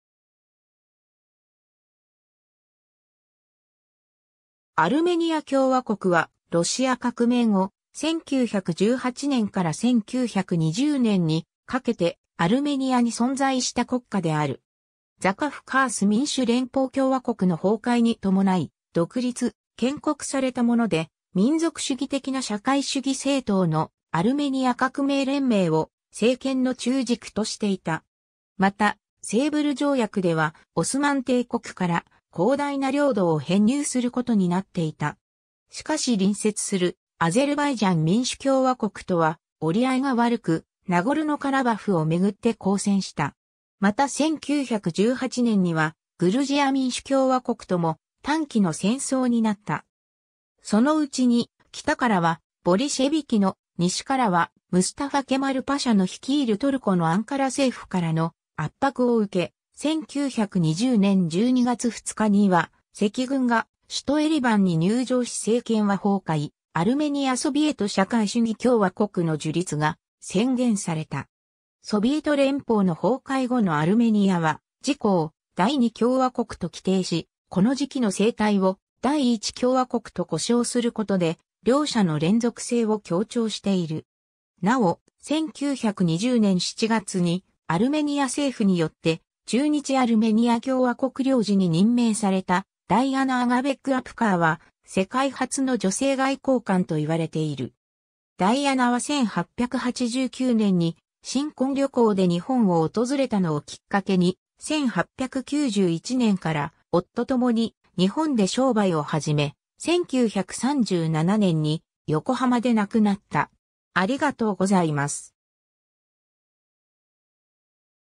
アルメニア第一共和国
アルメニア共和国（アルメニアきょうわこく、アルメニア語: Հայաստանի Հանրապետություն）は、ロシア革命後、1918年から1920年にかけてアルメニアに存在した国家である。
ザカフカース民主連邦共和国の崩壊にともない独立・建国されたもので、民族主義的な社会主義政党のアルメニア革命連盟（ダシュナク党）を政権の中軸としていた。
またセーヴル条約ではオスマン帝国から広大な領土を編入することになっていた。
 しかし隣接するアゼルバイジャン民主共和国とは折り合いが悪く、ナゴルノ・カラバフをめぐって交戦した。
また1918年にはグルジア民主共和国とも短期の戦争になった。
そのうちに北からはボリシェビキの、西からはムスタファ・ケマル・パシャの率いるトルコのアンカラ政府からの圧迫を受け、1920年12月2日には赤軍が首都エレバンに入城し政権は崩壊、アルメニア・ソビエト社会主義共和国の樹立が宣言された。
 ソビエト連邦の崩壊後のアルメニアは、自己を「第二共和国」と規定し、この時期の政体を「第一共和国」と呼称することで、両者の連続性を強調している。
 なお、1920年7月にアルメニア政府(首相ハモ・オハンジャニャン)によって駐日アルメニア共和国領事に任命されたダイアナ・アガベッグ・アプカー（Diana Agabeg Apcar、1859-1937）は、世界初の女性外交官と言われている。
ダイアナは1889年に新婚旅行で日本を訪れたのをきっかけに、1891年から夫ともに日本で商売を始め、1937年に横浜で亡くなった。